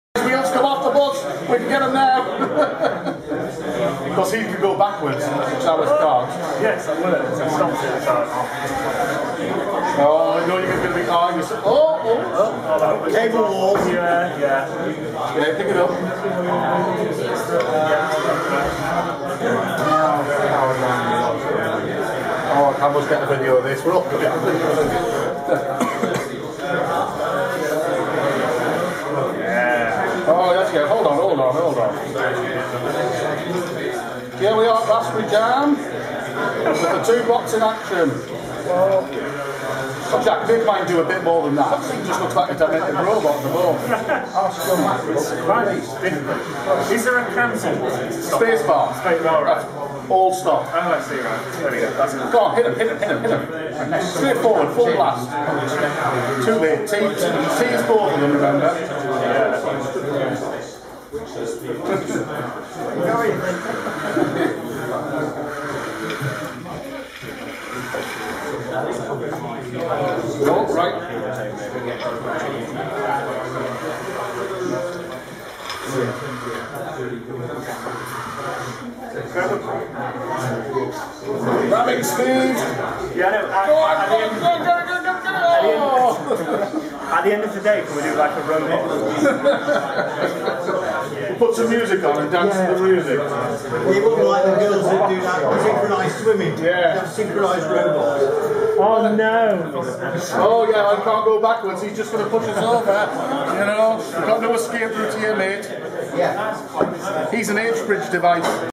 Manchester Raspberry Jam - Robot Wars (Video 2)
2 Robots created using the Raspberry Pi computer, demonstrated at Manchester Raspberry Jam 5 on 20th October 2012.

The robots are controlled using scratch and were built and programmed by Simon Walters

Manchester Raspberry Jam is a monthly event, organised by Ben Nuttall hosted at Manchester Digital Laboratory for more information about the event, which anyone is welcome to attend, for more information about the computer